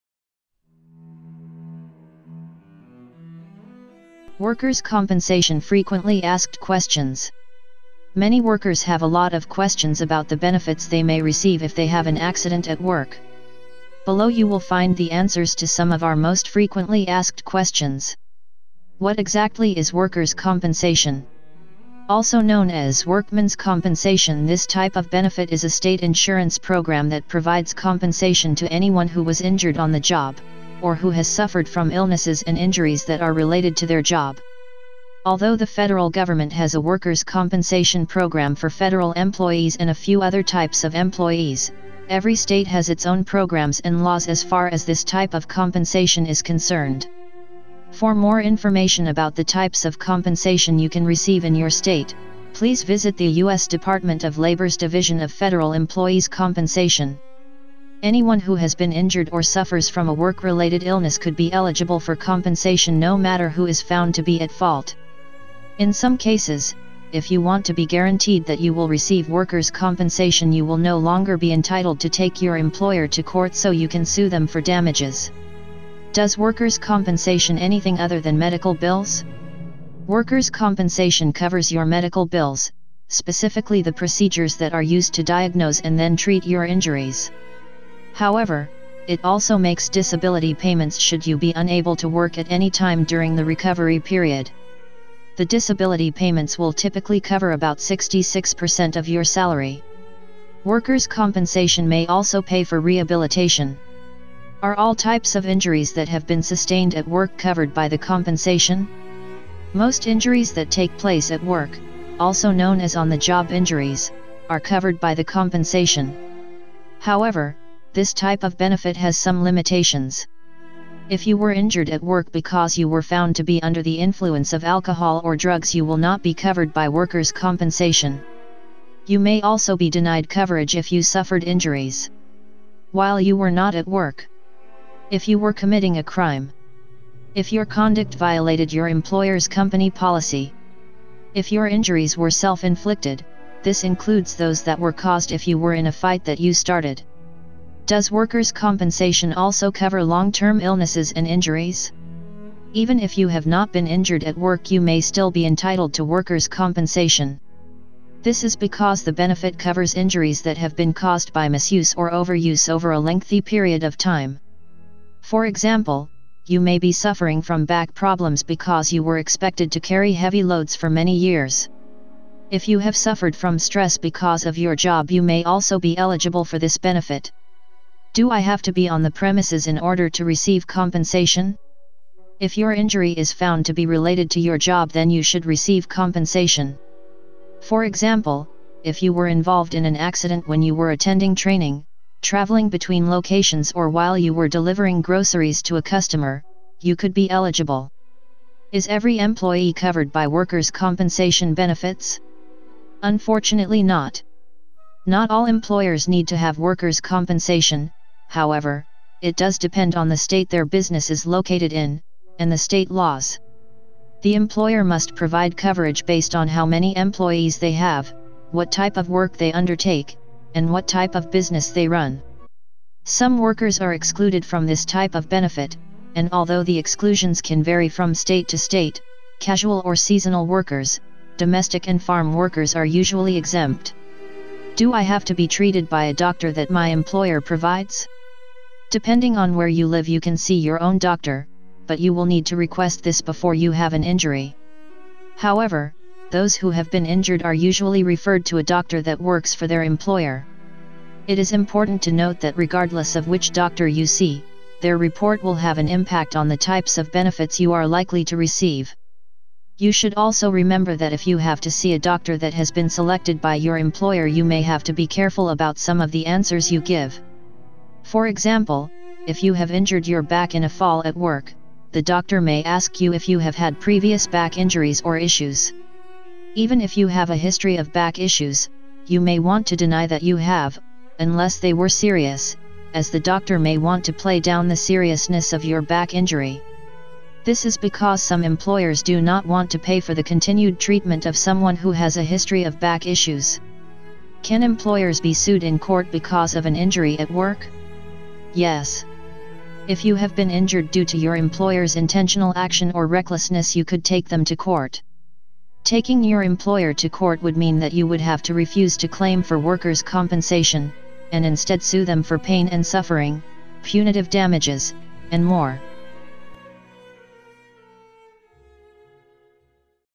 Workers' Compensation Frequently Asked Questions (FAQ)
Many workers have a lot of questions about the benefits they may receive if they have an accident at work. Below you will find the answers to some of our most frequently asked questions:

What exactly is workers’ compensation?
Also known as ‘Workman’s Compensation’ this type of benefit is a state insurance program that provides compensation to anyone who was injured on the job, or who has suffered from illnesses and injuries that are related to their job.
Although the federal government has a workers’ compensation program for federal employees and a few other types of employees, every state has its own programs and laws as far as this type of compensation is concerned. For more information about the types of compensation you can receive in your state, please visit the US Department of Labor's Division of Federal Employees' Compensation
Anyone who has been injured or suffers from a work-related illness could be eligible for compensation no matter who is found to be at fault. In some cases, if you want to be guaranteed that you will receive workers’ compensation you will no longer be entitled to take your employer to court so you can sue them for damages.

Does workers’ compensation anything other than medical bills?
Workers’ compensation covers your medical bills, specifically the procedures that are used to diagnose and then treat your injuries. However, it also makes disability payments should you be unable to work at any time during the recovery period. The disability payments will typically cover about 66% of your salary. Workers’ compensation may also pay for rehabilitation.

Are all types of injuries that have been sustained at work covered by the compensation?
Most injuries that take place at work (Also known as ‘On the job injuries’) are covered by the compensation. However, this type of benefit has some limitations. If you were injured at work because you were found to be under the influence of alcohol or drugs you will not be covered by workers’ compensation.

You may also be denied coverage if you suffered injuries:

While you were not at work
If you were committing a crime
◦If your conduct violated your employer’s company policy
◦If your injuries were self-inflicted (This includes those that were caused if you were in a fight that you started)

Does workers’ compensation also cover long-term illnesses and injuries?
Even if you have not been injured at work you may still be entitled to workers compensation. This is because the benefit covers injuries that have been caused by misuse or overuse over a lengthy period of time. For example, you may be suffering from back problems because you were expected to carry heavy loads for many years.
If you have suffered from stress because of your job you may also be eligible for this benefit.
Do I have to be on the premises in order to receive compensation?
If your injury is found to be related to your job then you should receive compensation. For example, if you were involved in an accident when you were attending training, traveling between locations or while you were delivering groceries to a customer, you could be eligible.

Is every employee covered by workers’ compensation benefits?

Unfortunately not. Not all employers need to have workers’ compensation, however, it does depend on the state their business is located in, and the state laws. The employer must provide coverage based on how many employees they have, what type of work they undertake, and what type of business they run.
Some workers are excluded from this type of benefit, and although the exclusions can vary from state to state, casual or seasonal workers, domestic and farm workers are usually exempt.

Do I have to be treated by a doctor that my employer provides?
Depending on where you live you can see your own doctor, but you will need to request this before you have an injury. However, those who have been injured are usually referred to a doctor that works for their employer.
It is important to note that regardless of which doctor you see, their report will have an impact on the types of benefits you are likely to receive. You should also remember that if you have to see a doctor that has been selected by your employer you may have to be careful about some of the answers you give. For example, if you have injured your back in a fall at work, the doctor may ask you if you have had previous back injuries or issues. Even if you have a history of back issues, you may want to deny that you have (Unless they were serious), as the doctor may want to play down the seriousness of your back injury. This is because some employers do not want to pay for the continued treatment of someone who has a history of back issues.